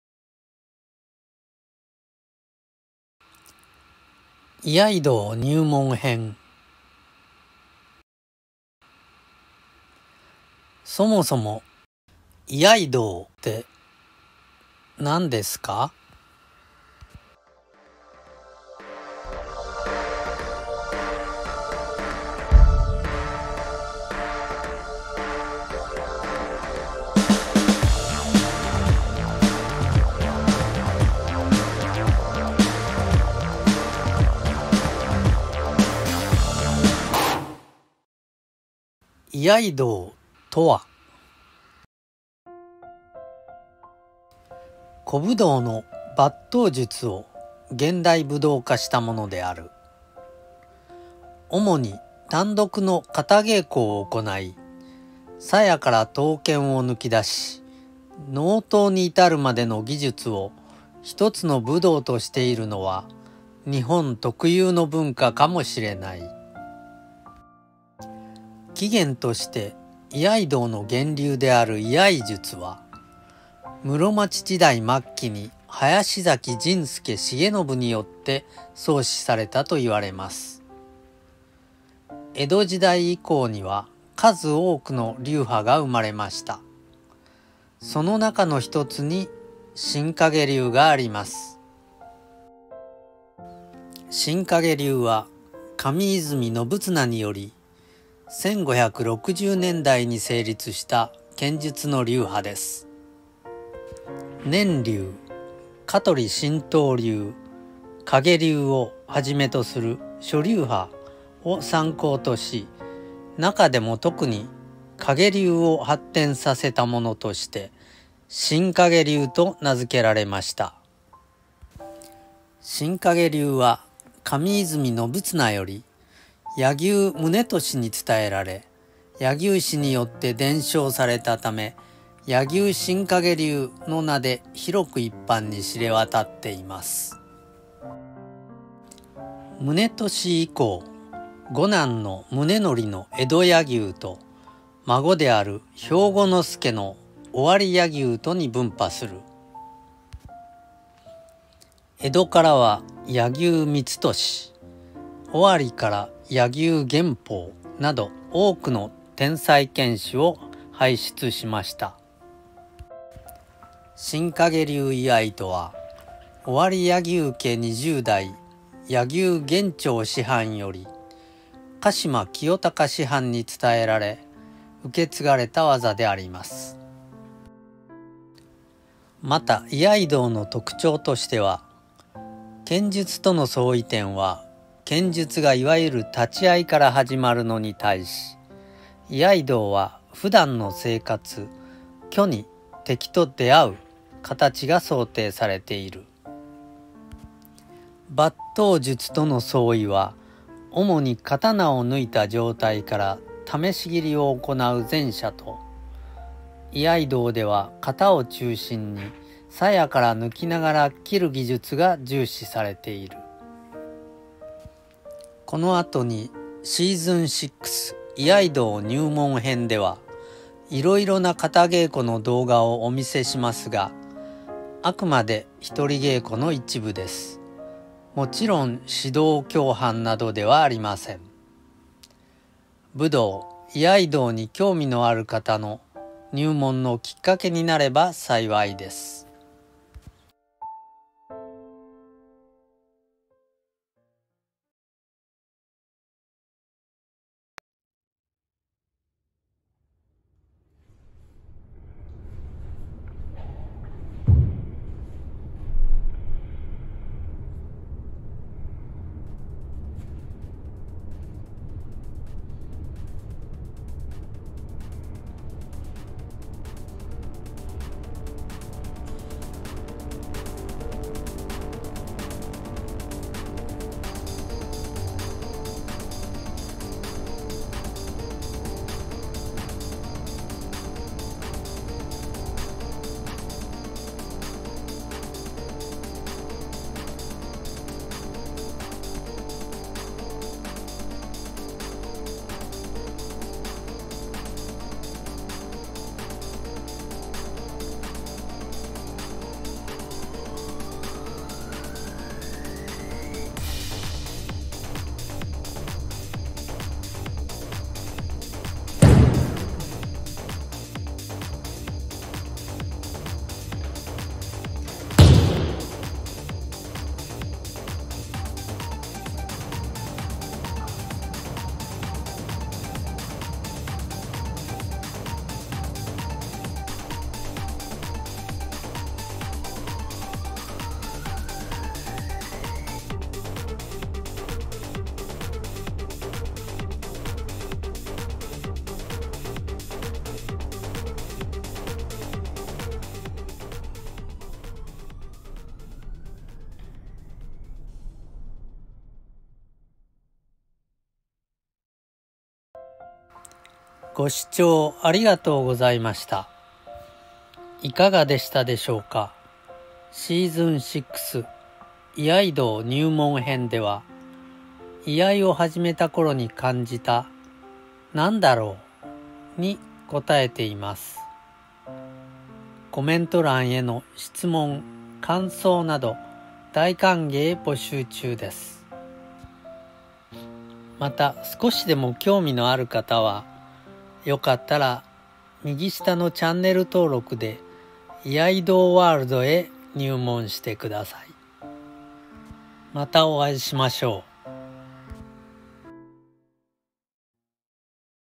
【新陰流居合】そもそも「居合道」て何ですか？ shinkageryu iai alone training
新陰流古流居合ひとり稽古記録
シーズン6．居合道入門編

そもそも「居合道」iaido とは？で始まる基礎知識、
武道や居合に興味ある方の入門のキッカケになれば幸いです

【目次】
00:00　オープニング
00:27　解説
04:02　稽古記録
05:52　エンディング